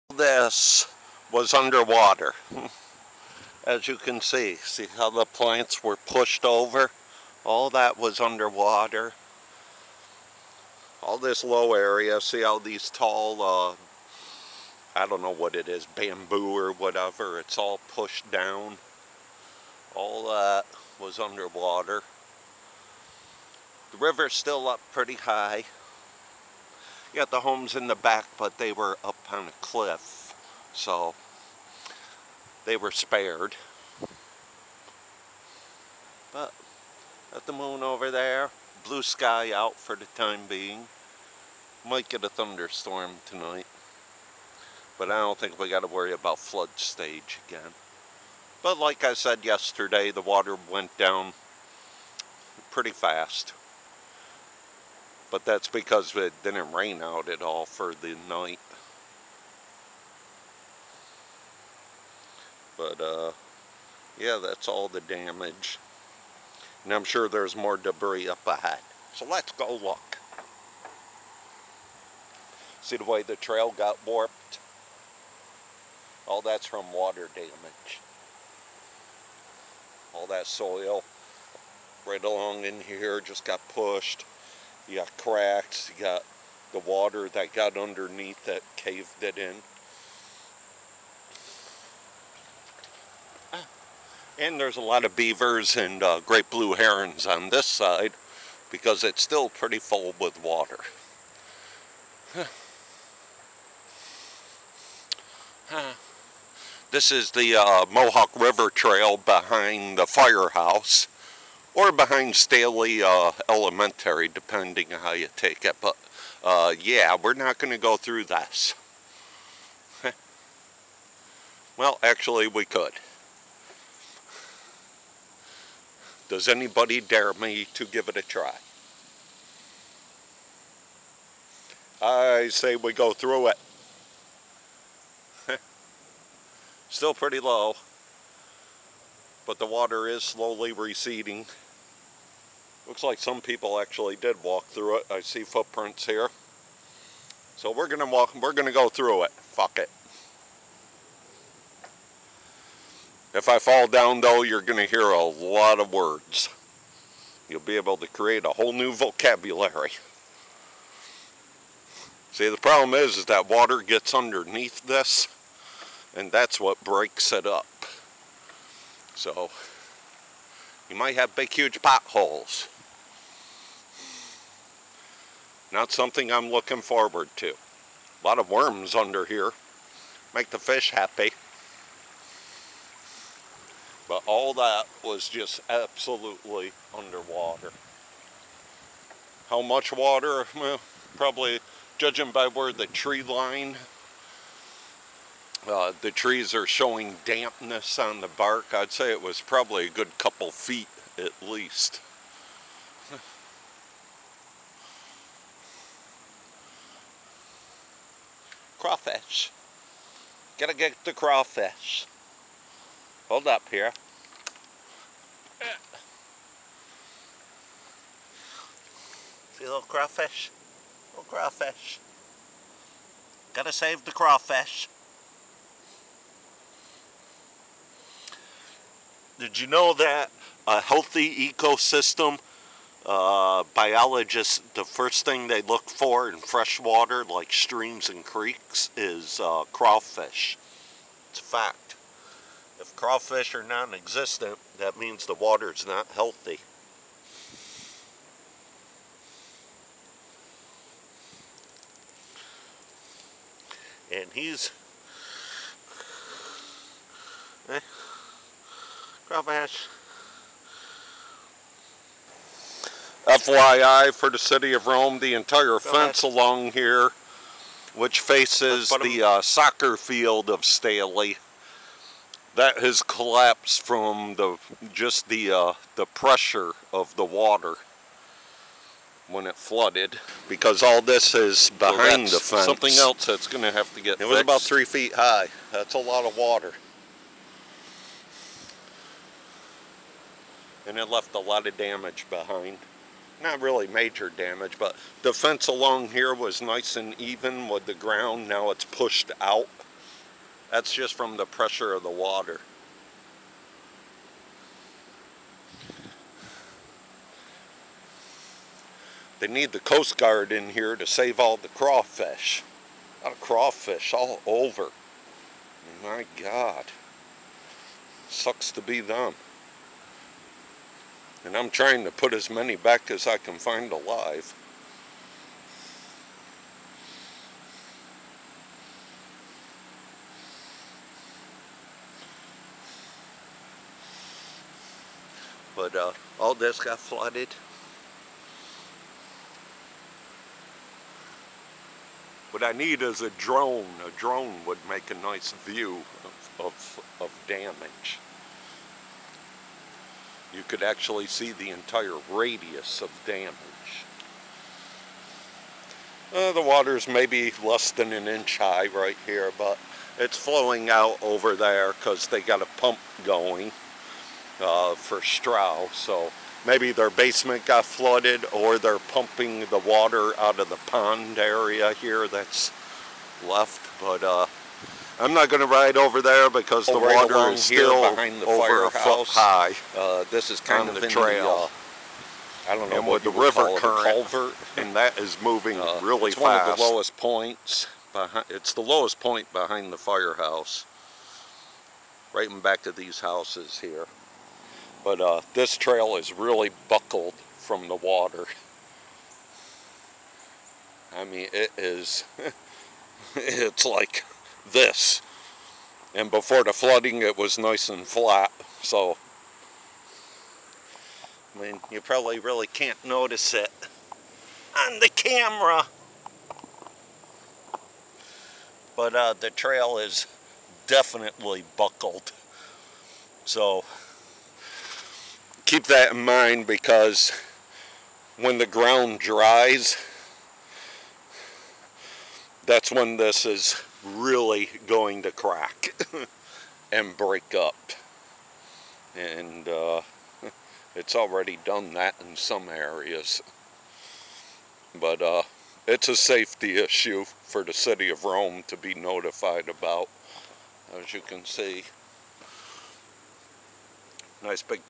Epic Flood Damage on Mohawk River
Well. A day after the flooding and turns-out some businesses on East Dominick(Savoy Restaurant) did receive damage from Mother Nature(or if you're a liberal...Damage caused by everything human), as did some homes.
FYI to City of Rome, NY: The Mohawk River trail, behind the firehouse, did receive some damage for about forty feet. The trail is warped & buckled from water damage. And much of the side has collapsed. Gonna need the engineering department to look-at-it & at least post a sign telling bikers to slow down(to prevent any lawsuit).
Water by Staley School on East Bloomfield street, is still pretty high, even though the water is receding. You can hear water bilge pumps behind Staley school. The water line near the playground at the school reached three feet! Enough power in that current to wash away any vehicle. And that's using the debris on the playground fence as a scale for water height. The Playground perimeter fence may need some repairs as well as the fence next to the soccer field, which the force of water collapsed.
Noticed a-lot of little fish(now bird food), along the trail as well as dozens of crayfish. Picked-up  a dozen-of'em and put'em into calm water. Most were deceased. Lotta sand now on the trail. Either Nature can remove-it over time, or Rome can have a small front loader, scrape the trail behind Staley school at a later date. Homes in front of the trail escaped damage, being they are much higher than the river. However, the officials should check the back-of-the-homes, for erosion damage or unstable land near the river bank.
Let this be a lesson, for the next time. Rome, New York should have a ready-supply of sandbags, or squabble with the new NY Governor, for funding to create better drainage.
Oddly, during a flood event, the flood gates of the Barge Canal, are closed. Not since last night, have they been shut. Maybe today. Maybe they decided that closing'em causes more flooding.
Oh: Today was a glorious sunset.